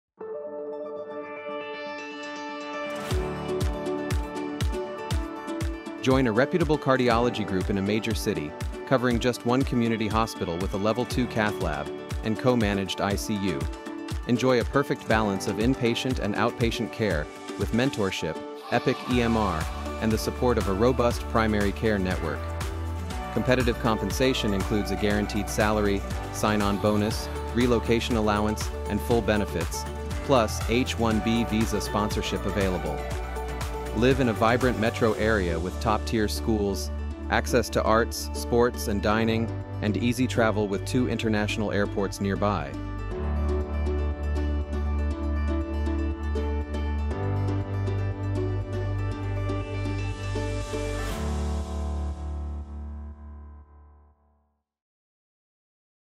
Cardiology Opportunity in Ohio
Join a reputable cardiology group in a major city, covering just ONE community hospital with a Level II Cath Lab and co-managed ICU. Enjoy a perfect balance of inpatient and outpatient care with mentorship, EPIC EMR, and the support of a robust primary care network. Competitive compensation includes a guaranteed salary, sign-on bonus, relocation allowance, and full benefits, plus H1-B visa sponsorship available. Live in a vibrant metro area with top-tier schools, access to arts, sports, and dining, and easy travel with two international airports nearby!